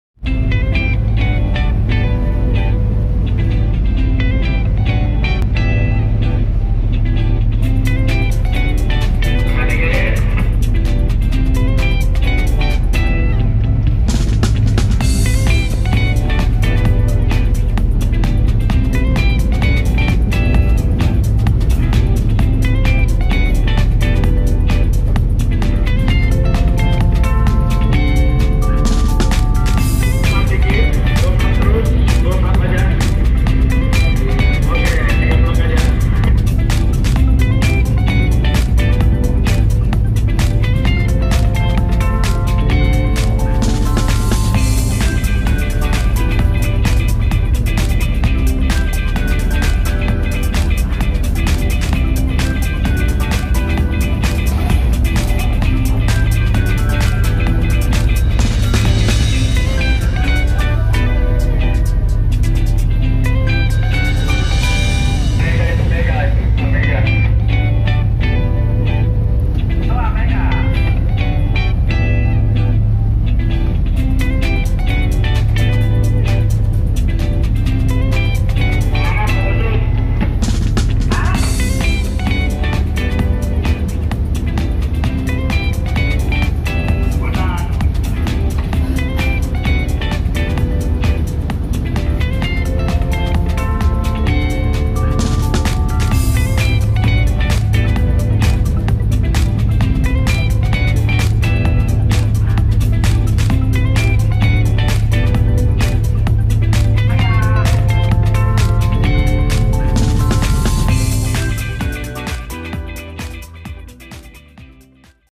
Offload Cargo Whilst Maneuvering Maintaining Position
Offload cargo to platform is one of so many tasks in offshore field, here's the video showing offload cargo in offshore location.